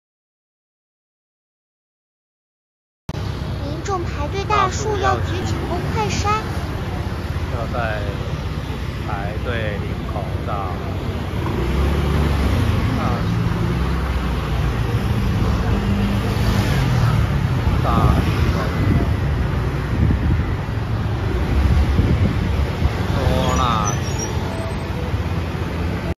台南民眾每日藥局排隊搶快篩 （每日限量78組）台南崇德路上大樹藥局111年5月11日
台南民眾每日藥局排隊搶快篩 台南崇德路上大樹藥局
每日供應限量78組111年5月11日